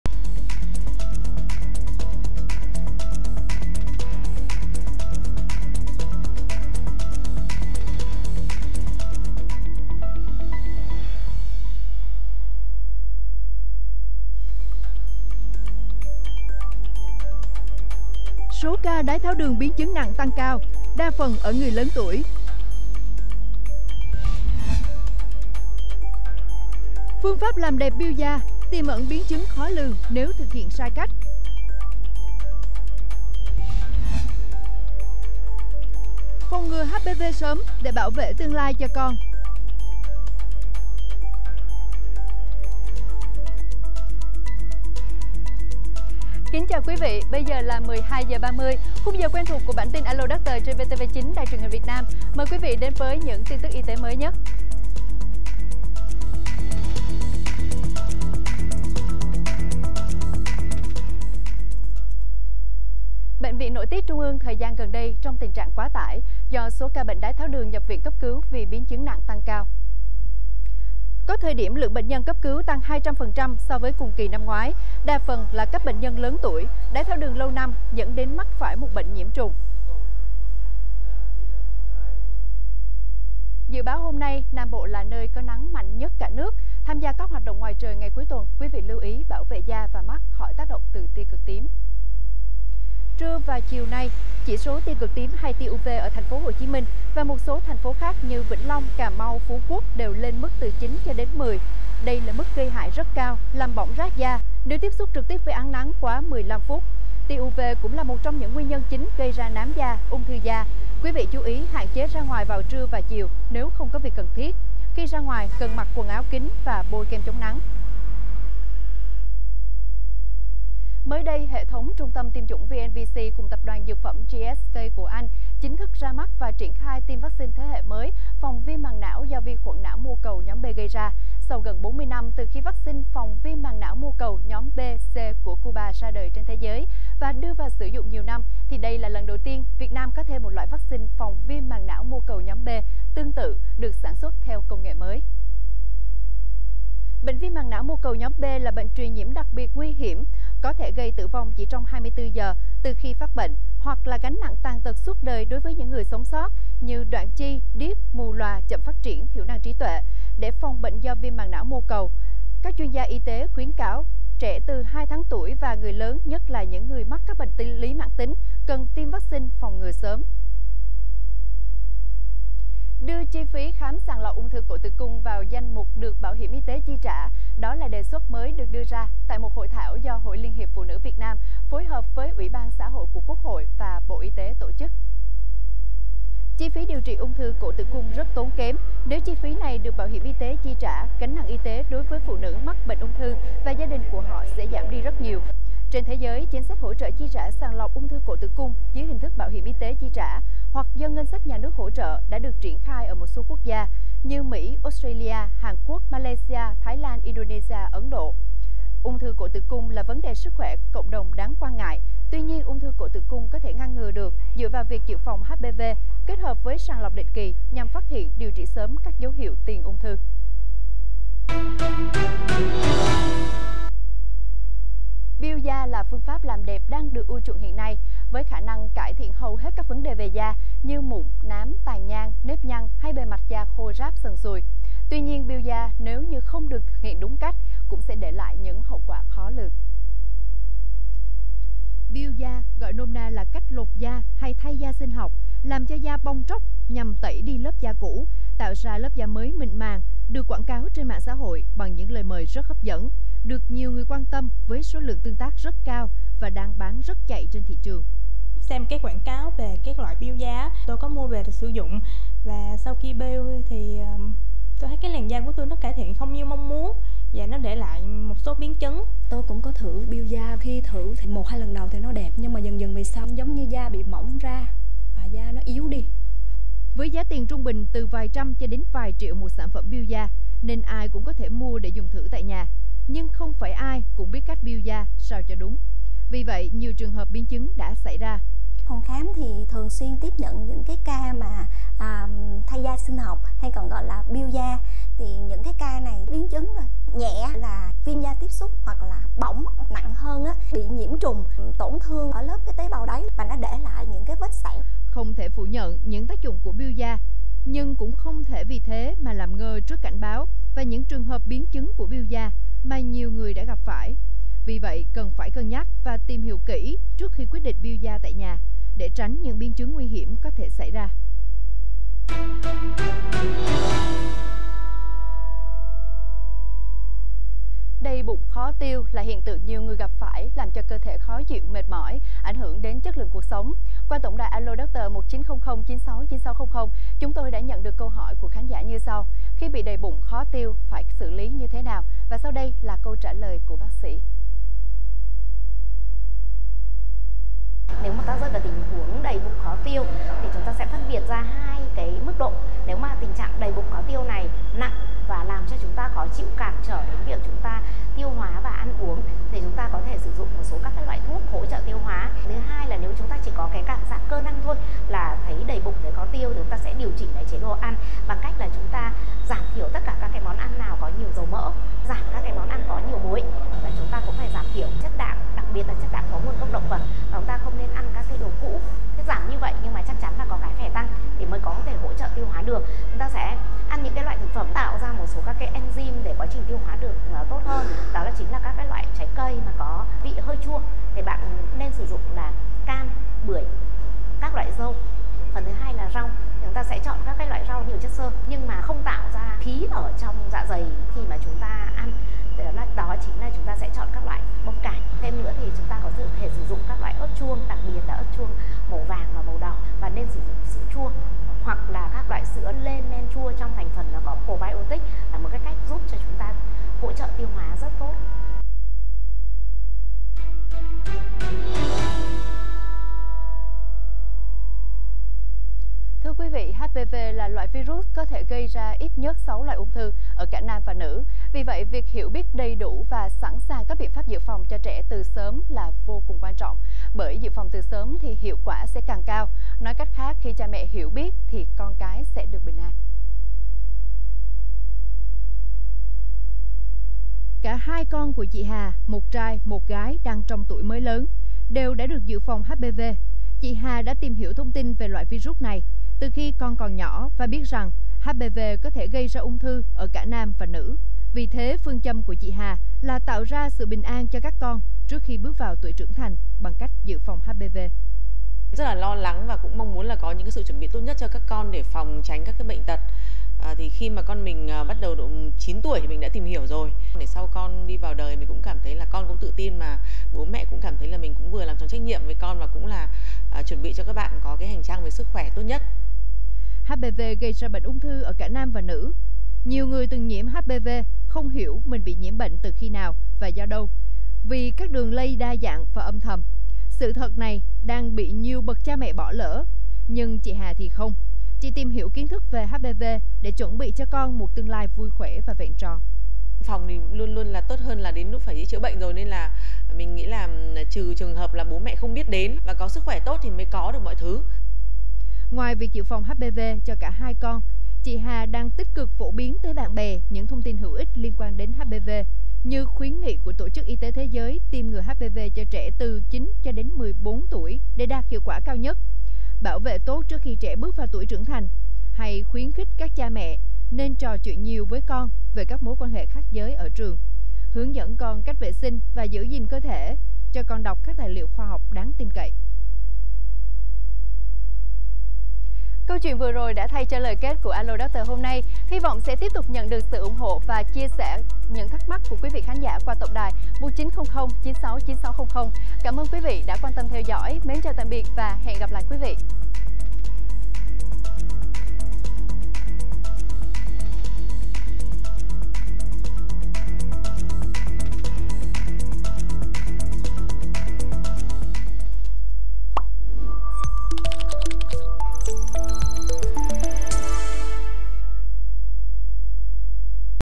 Số ca đái tháo đường biến chứng nặng tăng cao. Đa phần ở người lớn tuổi | Alo Doctor
Số ca đái tháo đường biến chứng nặng tăng cao. Đa phần ở người lớn tuổi

Phương pháp làm đẹp peel da - tiềm ẩn biến chứng khó lường nếu thực hiện sai cách
Phòng ngừa HPV sớm , để bảo vệ tương lai cho con